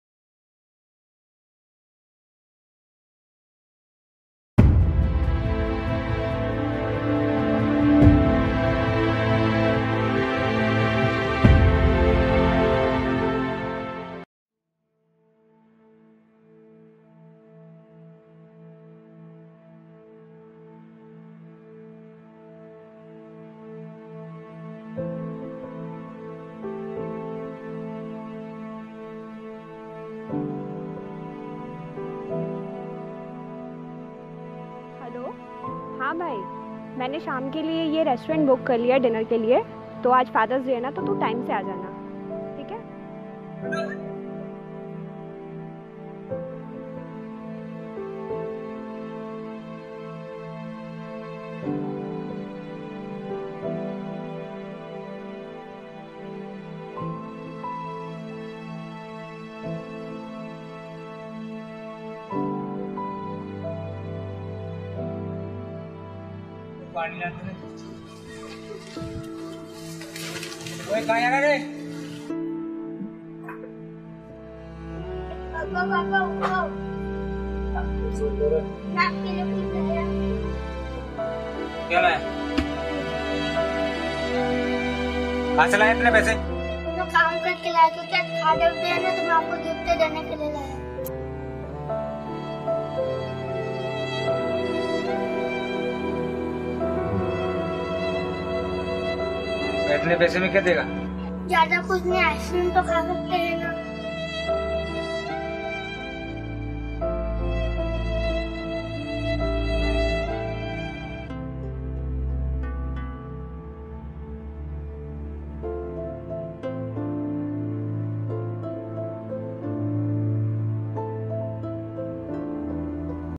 Student's Assignment- Short Movie
Let us reflect gratitude for our fathers, ever willing to sacrifice his own comforts for that of his family, works hard selflessly to provide for the necessities of his family without any complaints. He effortlessly plays the roles- The pillar of strength, Source of motivation, Backbone of the Family..
Here's s a movie dedicated to all the lovely Fathers out there.